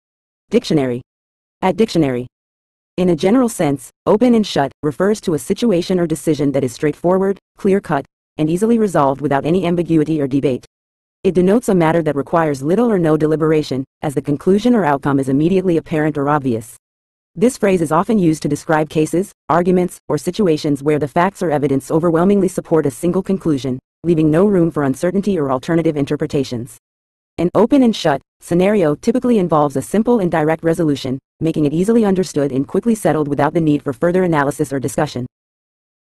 Open-and-shut Meaning In English
In a general sense, "open-and-shut" refers to a situation or decision that is straightforward, clear-cut, and easily resolved without any ambiguity or debate. It denotes a matter that requires little or no deliberation, as the conclusion or outcome is immediately apparent or obvious. This phrase is often used to describe cases, arguments, or situations where the facts or evidence overwhelmingly support a single conclusion, leaving no room for uncertainty or alternative interpretations. An "open-and-shut" scenario typically involves a simple and direct resolution, making it easily understood and quickly settled without the need for further analysis or discussion.